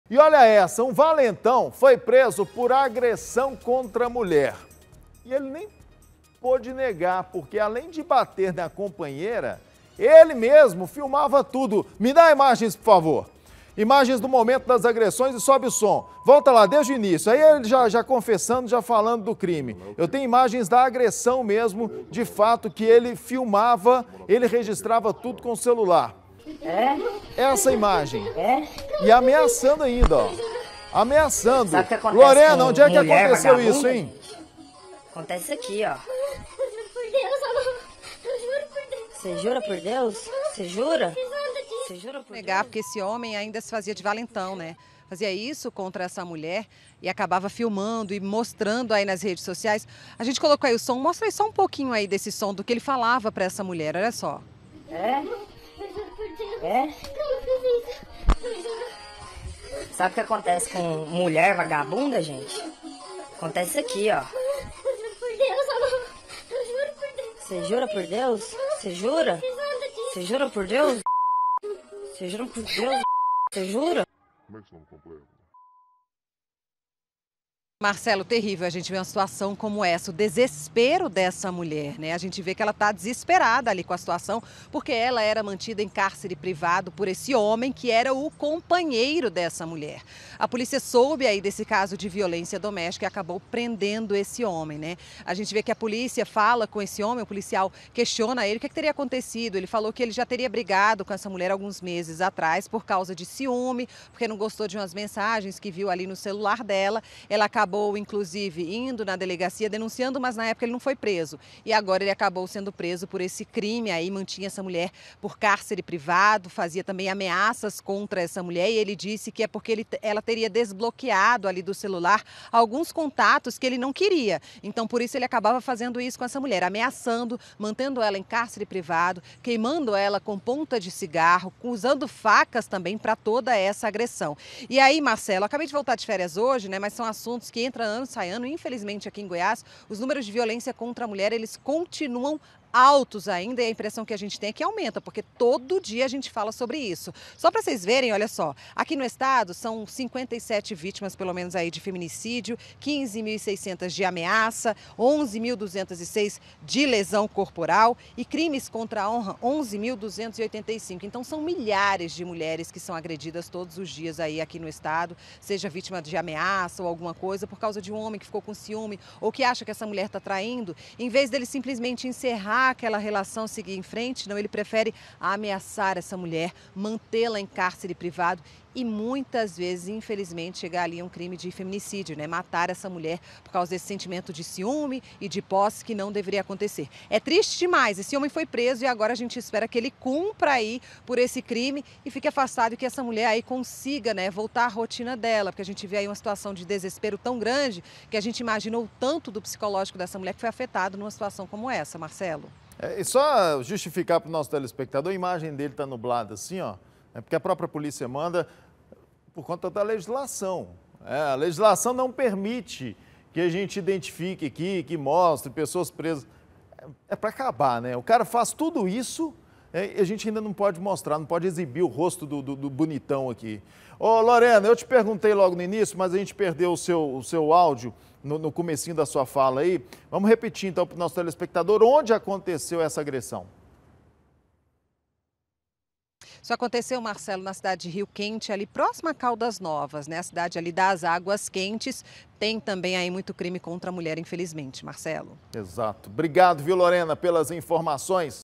COVARDÃO: HOMEM AGRIDE MULHER E FILMA
UM VALENTÃO FOI PRESO POR AGRESSÃO CONTRA A MULHER E ELE NEM PÔDE NEGAR, PORQUE ALÉM DE BATER NA COMPANHEIRA, ELE FILMAVA TUDO.